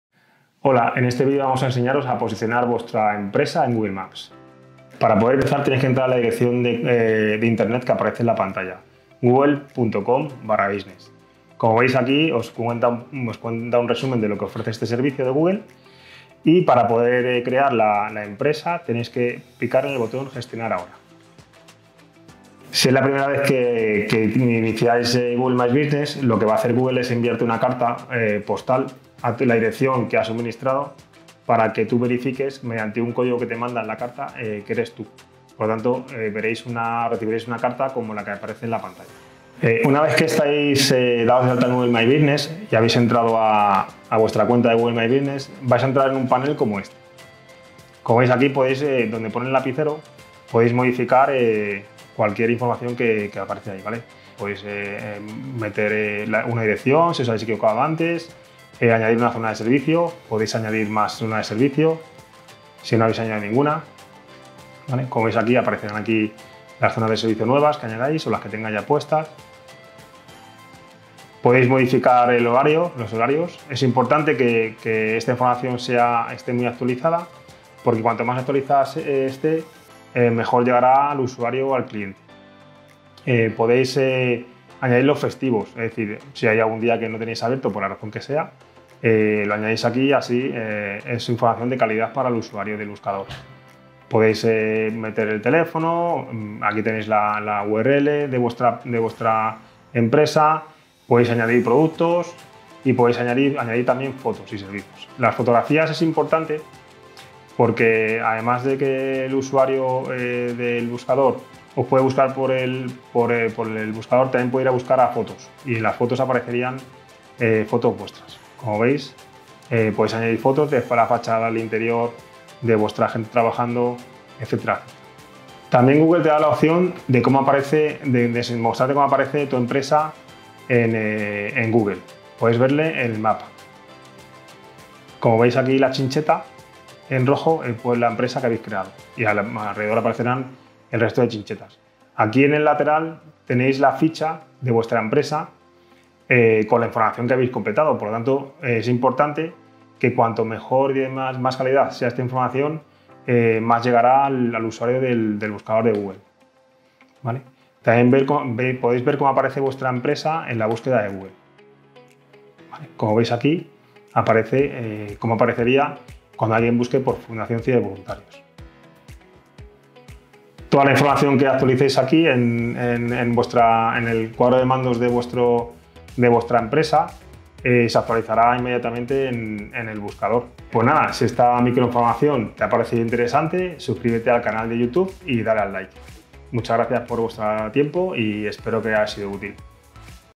Cómo poner tu negocio en Google Maps con Google My Business
💻  ¿Quieres que tu negocio o empresa aparezca en Google Maps? ¿Te gustaría saber cómo utilizar Google My Business?

📽️ En esta nueva Microformación de Cibervoluntarios, el cibervoluntario Israel Díaz te explica con sencillez todo lo que necesitas saber sobre esta plataforma.